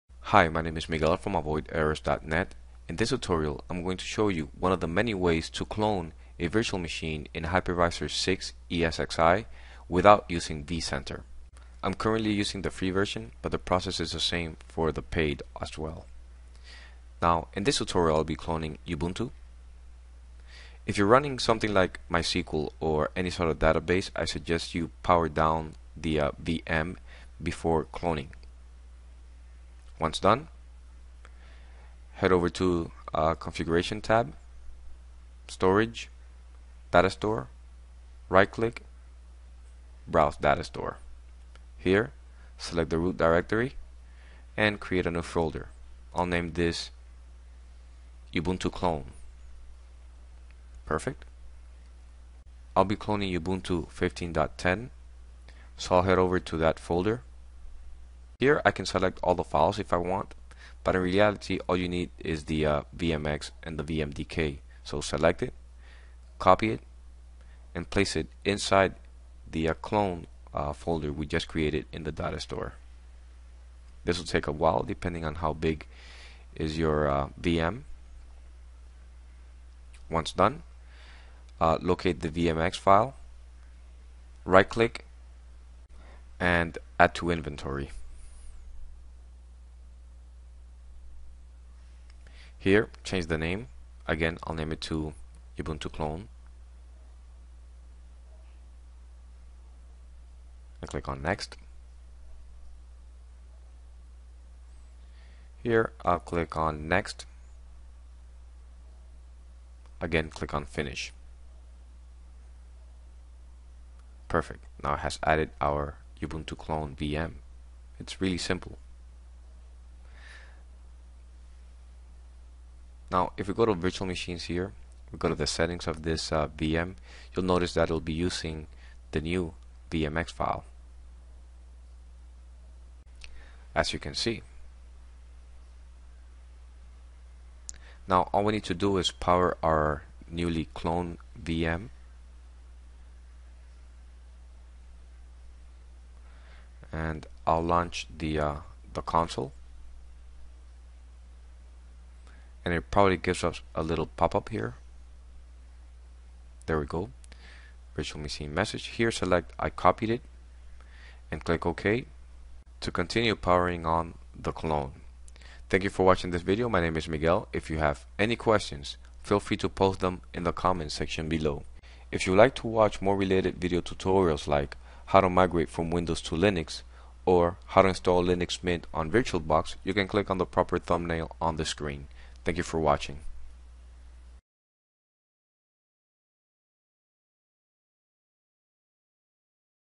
Clone a Virtual Machine Without vCenter in ESXI 6 Free Edition
This video tutorial provides instructions on how to clone a virtual machine in ESXI 6 Free edition.

Cloning a Virtual Machine in ESXI free edition is a feature of vCenter server. But there is an alternative way to clone without vCenter.